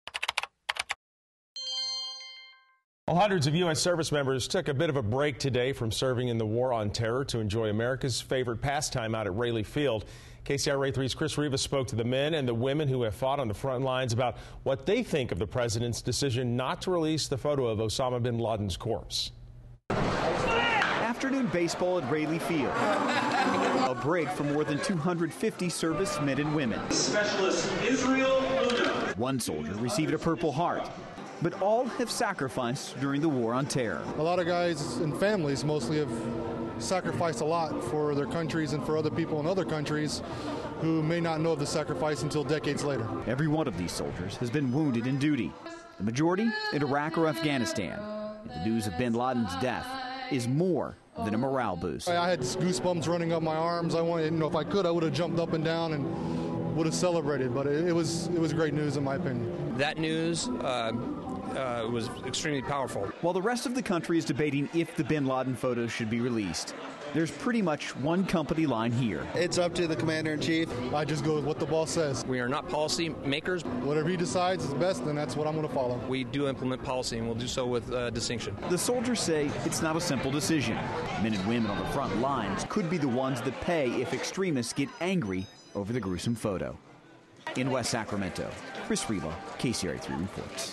Soldiers Take A Break To Enjoy Some Baseball
KCRA 3's Chris Riva spoke with military service men and women at Raley Field about what they think of the President's decision to not release the photo of Osama Bin Laden's corpse.